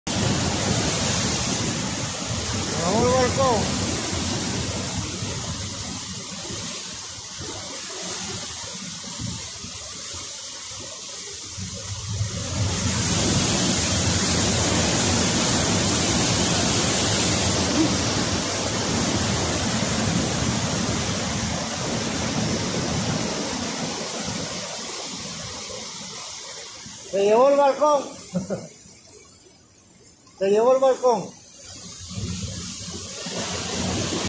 El edificio arrasado por las olas en Tenerife se construyó sobre el mar
El vecino, móvil en mano, ya intuía que algo serio podía ocurrir. Hasta que esta ola gigante lo confirma. Atentos a la fuerza con la que el agua golpea al edificio y la facilidad con la que se lleva por delante la barandilla y todo lo que había en el balcón. Hoy hemos vuelto al edificio y esto es lo que nos hemos encontrado. El apartamento del primero está destrozado y no es el único. Y no nos extraña si vemos desde el aire dónde está situado. El edificio se construyó directamente sobre el mar.

Fuente: ATLAS